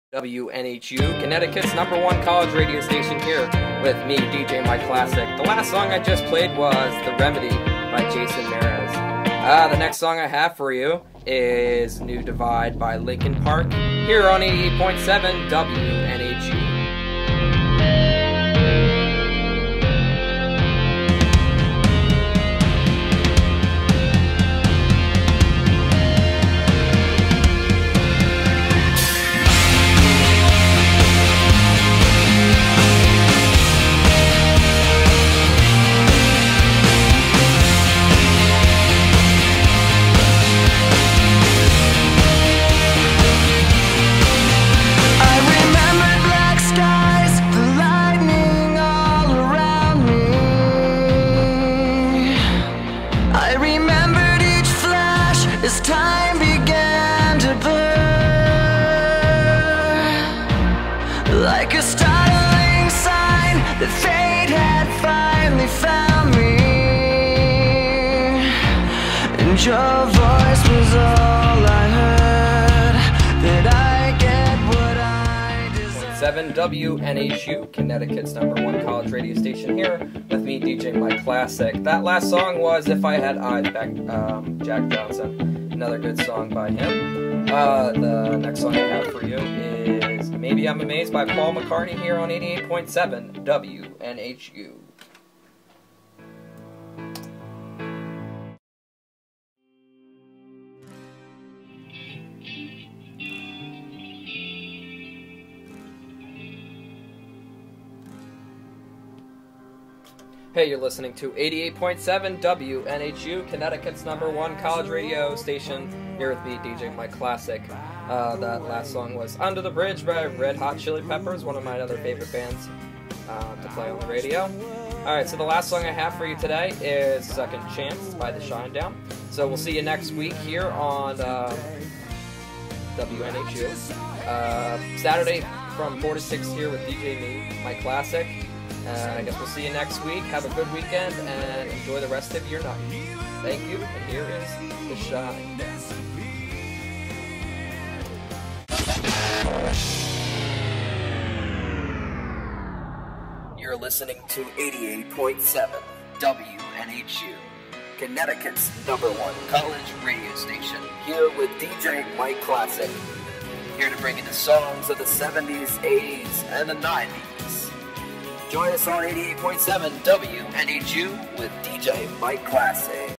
Radio Broadcast
I'm a radio Dj for my school and this is my broadcast that day